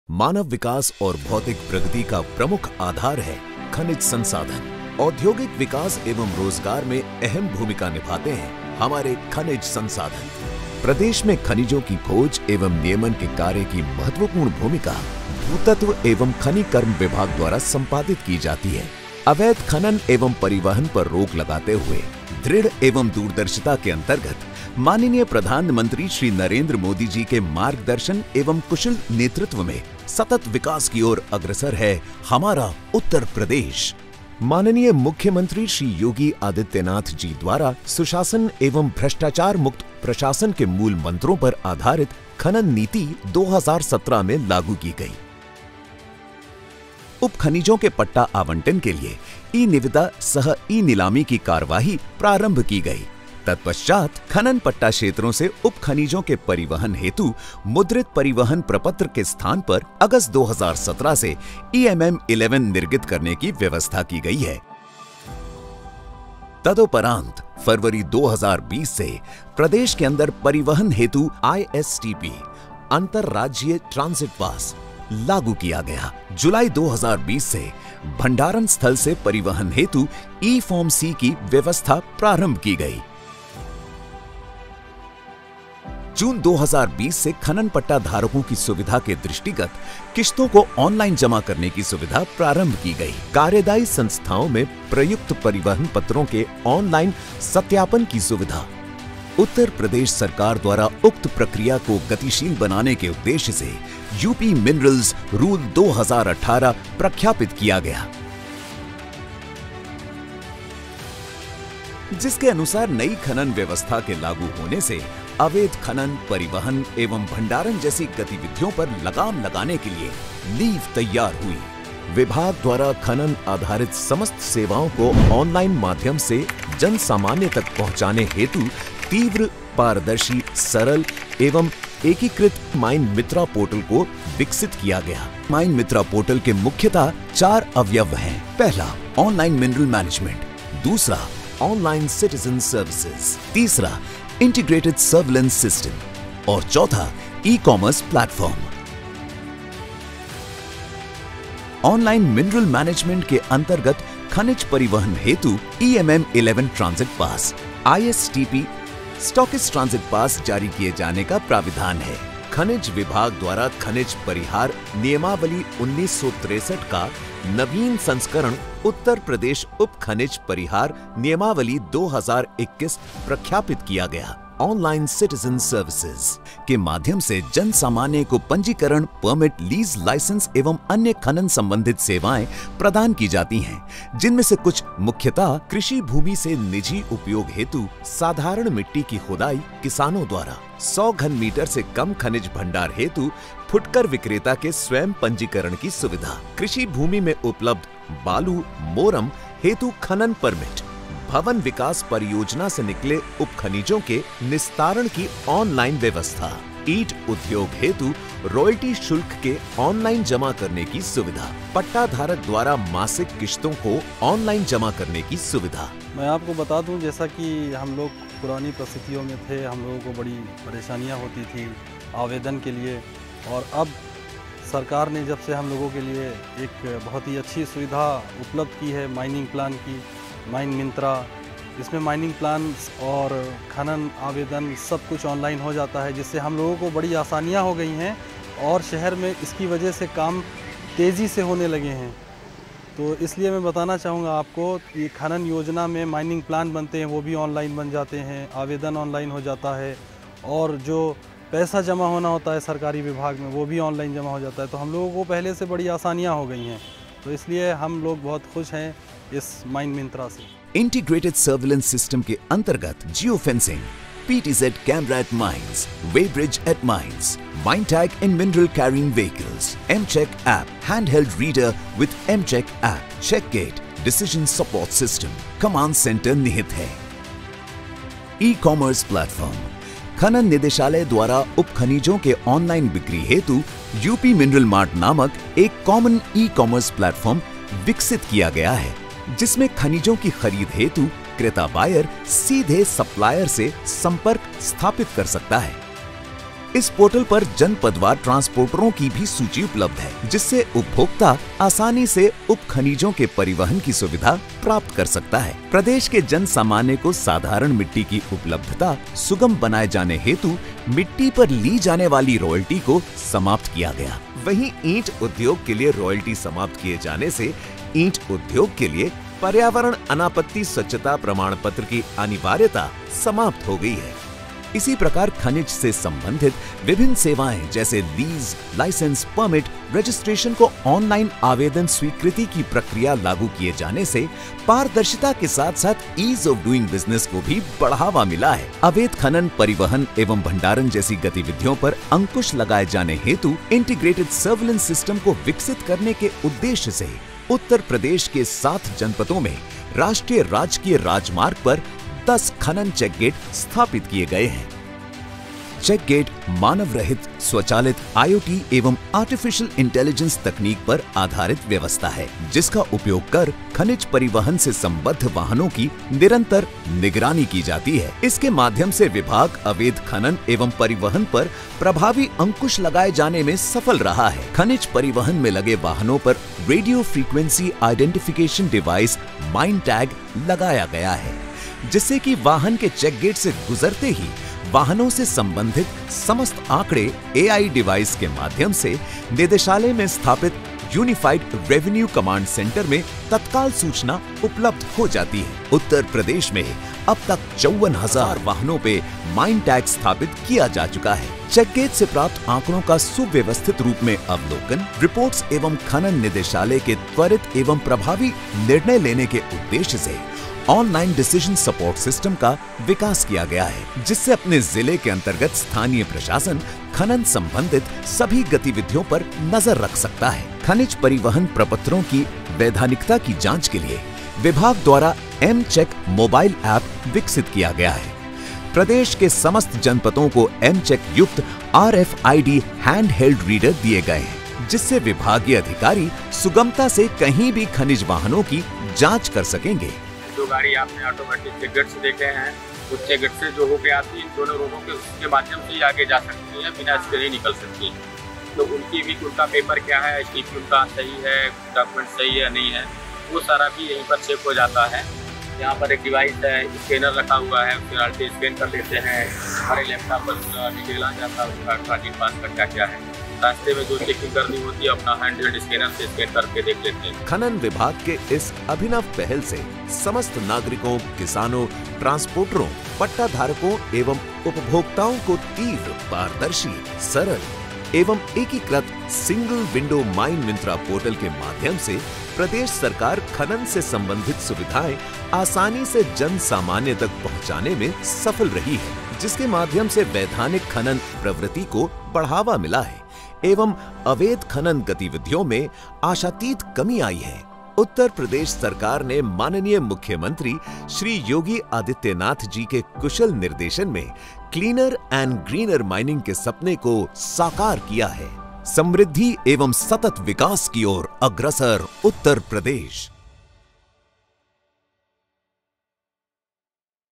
MINEMITRA Documentary
An end-to-end mineral management system, bringing all citizen-centric services & mining surveillance under one umbrella for ease of doing business & better e-governance